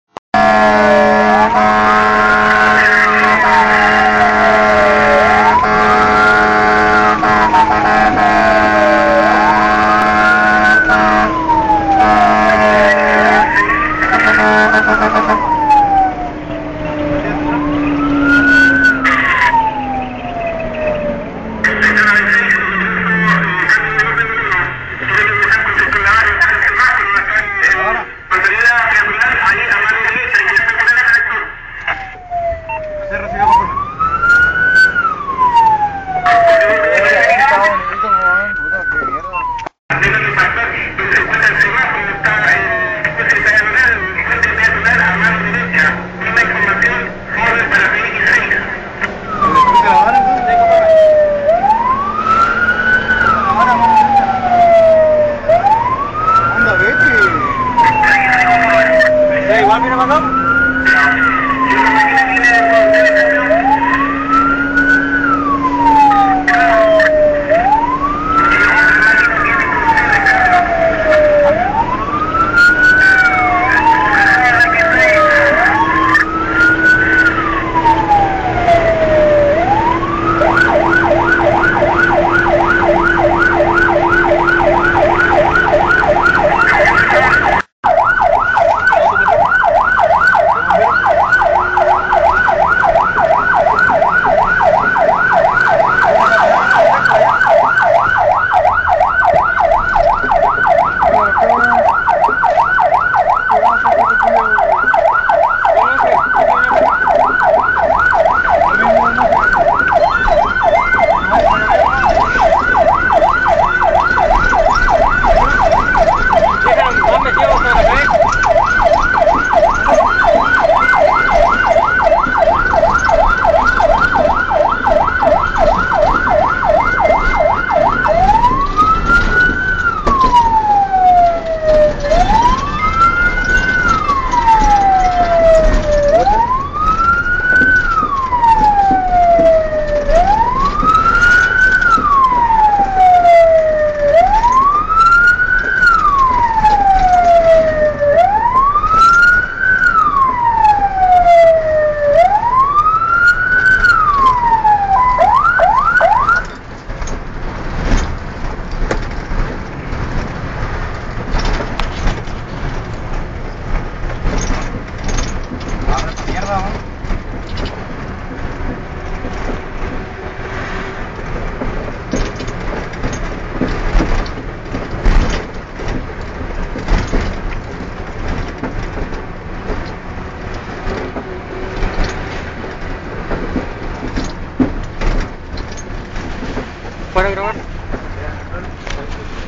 Alerta Máxima Puerto Montt
Gran ola de incendios se dejo ver en Puerto Montt, a fines de Febrero a causa de las altas temperaturas y descuidos de los ciudadanos.